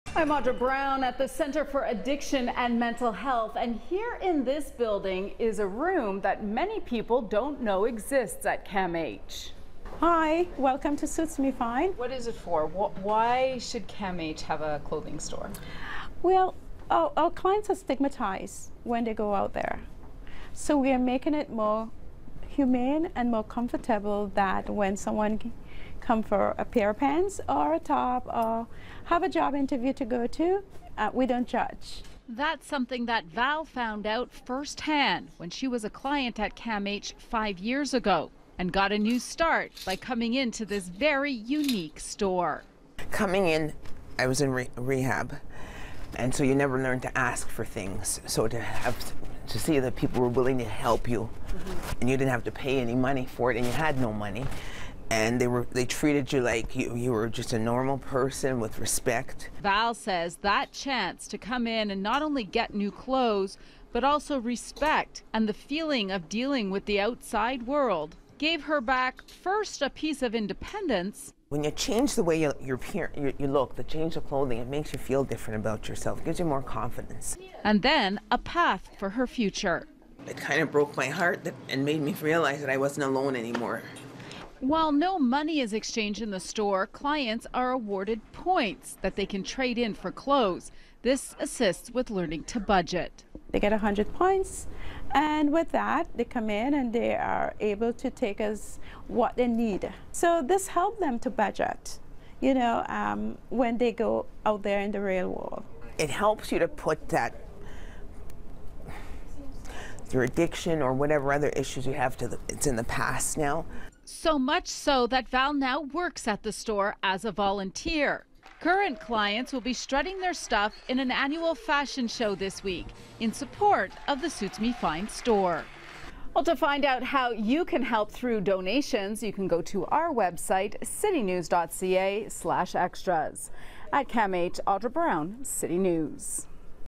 CAMH store outfits clients for new life
A special store at CAMH helps recovering clients dress for interviews and relearn important life skills with dignity. Audra Brown with how the store is helping prepare clients for a new life sans judgement.